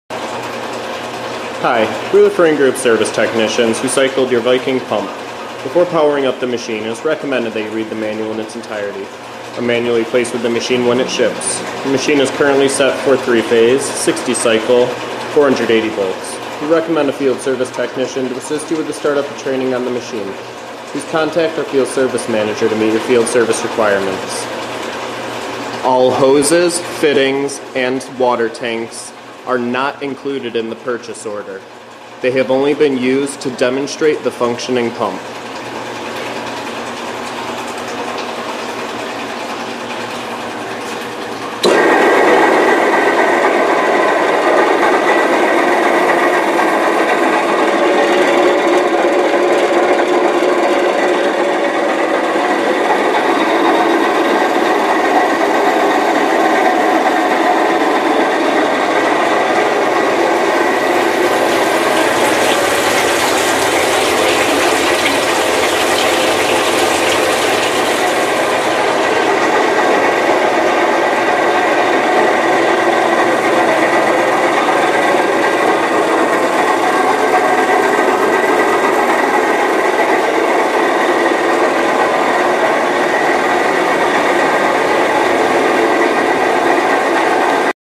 Viking Pump Positive KK224A 5F9869
Viking Model KK224A, positive displacement, heavy duty jacketed internal gear pump rated from 10 to 100 gallons per minute - depending on materials and application. Maximum operating pressure of 100 psi. and viscosity range of 28 SSU to 250,000 SSU and operating temperature range from -60 deg F to 450 deg F.  Cast iron construction low pressure  jacketed pump can then be piped with heat transfer oil, hot water or steam to precisely control the temperature. and are ideal for temperature sensitive applications such as chocolate.  Equipped with 2" OD sanitary in/outlets, and a 3Hp drive with on/off controls.  Mounted on 48" long x 14" wide base frame.